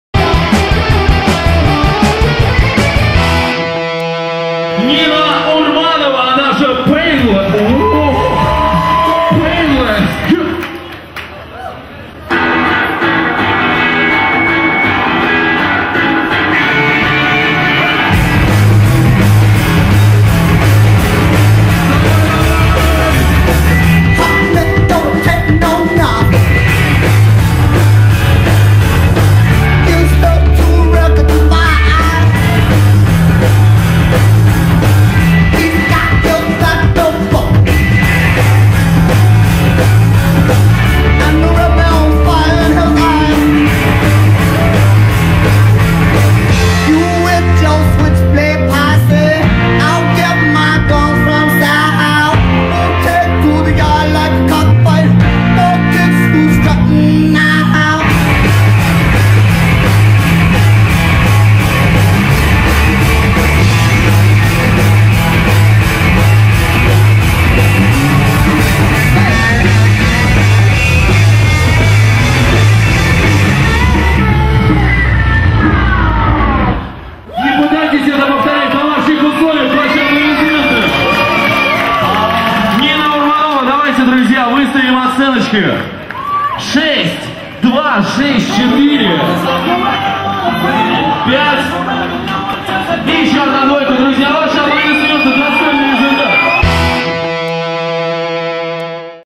Nina Painless - Air Guitar Russia Championships 2009 Moscow
Participant №3
Scores: 6+2+6+4+5=23
Place: 4 (Shared with Pare (Participant №10))